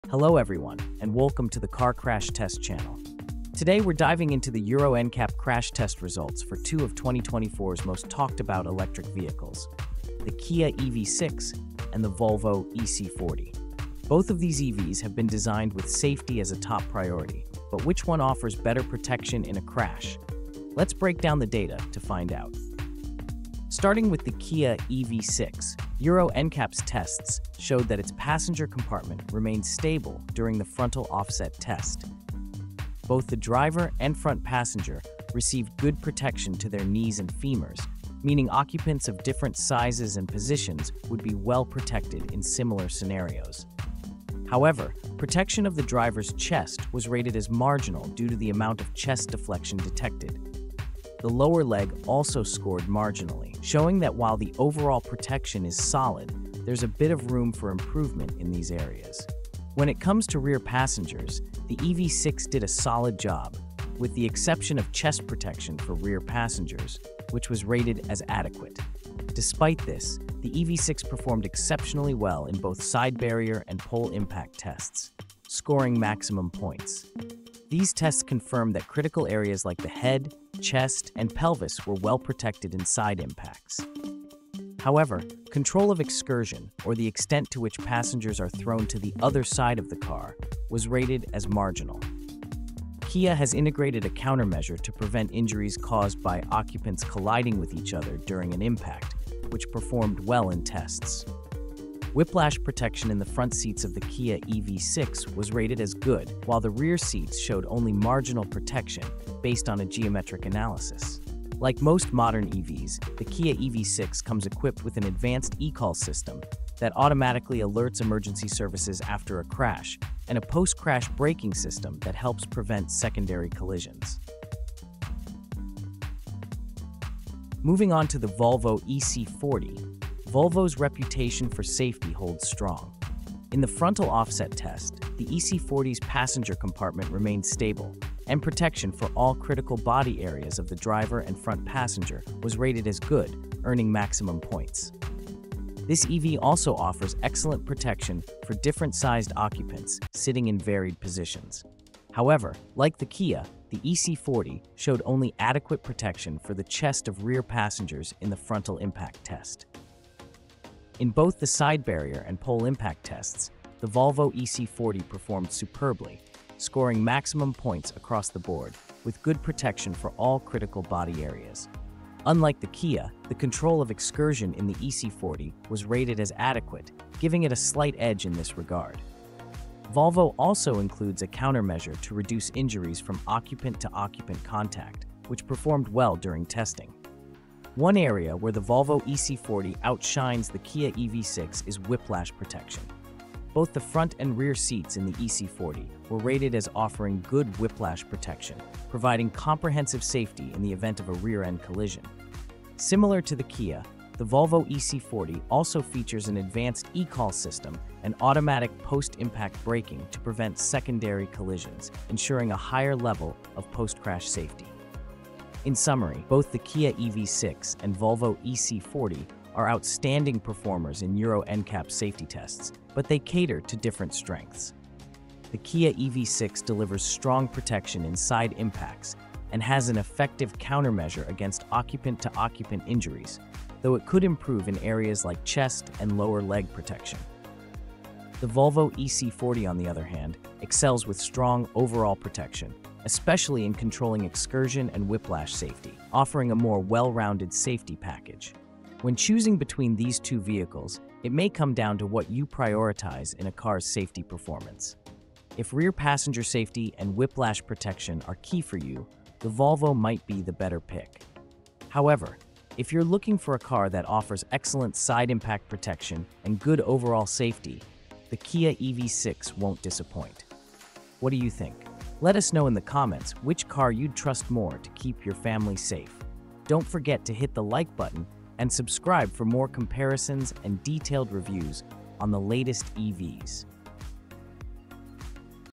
Kia EV6 vs Volvo EC40 (C40 Recharge) -EV Safety Showdown 2024
Kia EV6 vs. Volvo EC40 Euro NCAP Crash Test Comparison: Which is Safer?

In this video, we compare the Euro NCAP crash test results of two leading electric vehicles—the 2024 Kia EV6 and Volvo EC40. Find out how both cars performed in frontal offset, side barrier, pole impact tests, and much more! Which EV offers better safety for you and your family?